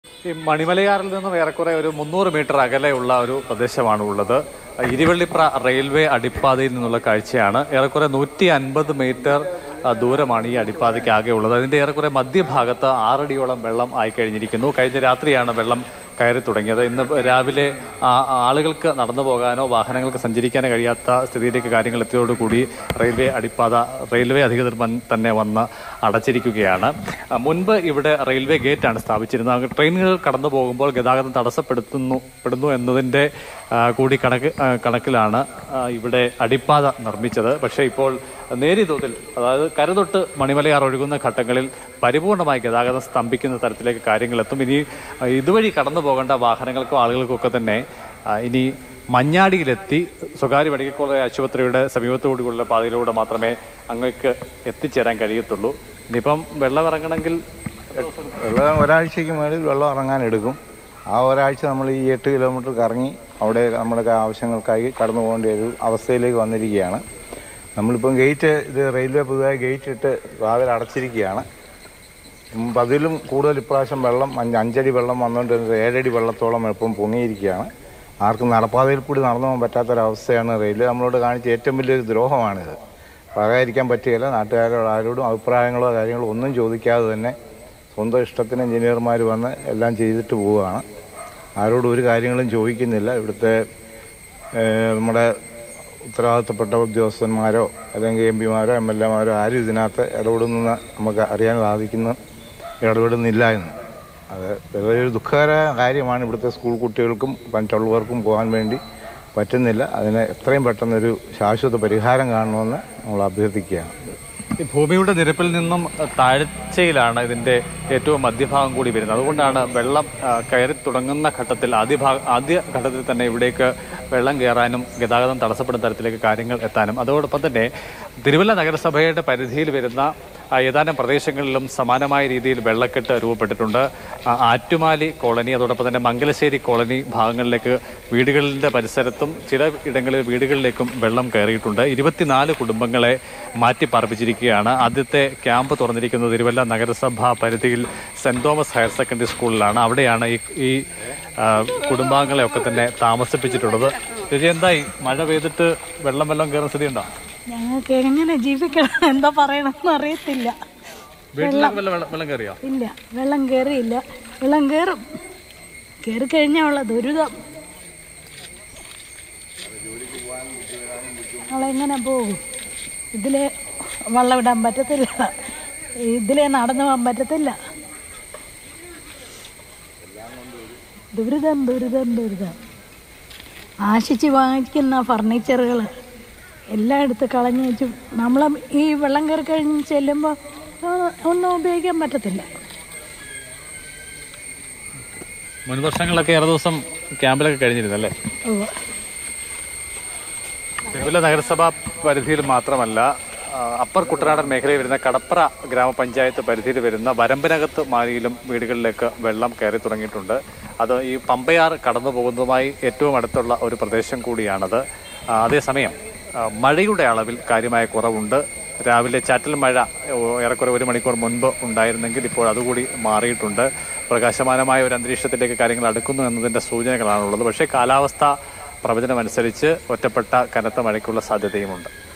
'ദുരിതം തന്നെ.. ഈ വെള്ളം കയറിക്കഴിഞ്ഞാല്‍ ആശിച്ച് വാങ്ങുന്ന ഫര്‍ണിച്ചറൊന്നും ഉപയോഗിക്കാന്‍ പറ്റില്ല'
Happy viewing!